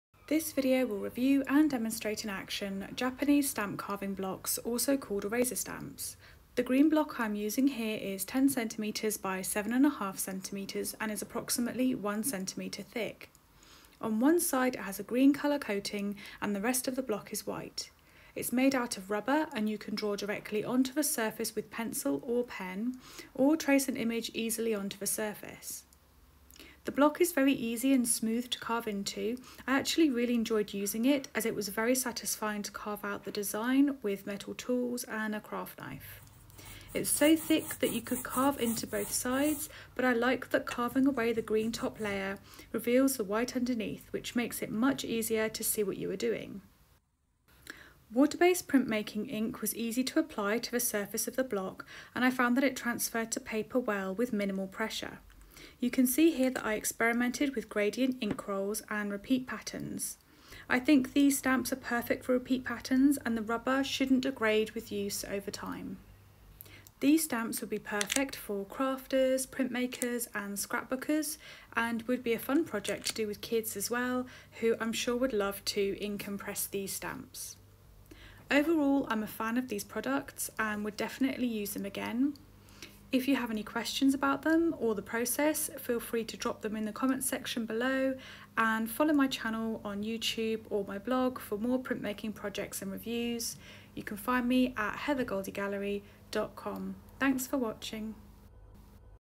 Rubber stampmaking - product review and project demonstration
This video demonstrates in action, and reviews, Japanese stamp carving blocks, also known as eraser stamps. The video shows how to transfer an image, carve a stamp for printmaking, adding ink to the block, and pressing into repeat patterns. A full written blog can be found on my website at heathergoldiegallery.com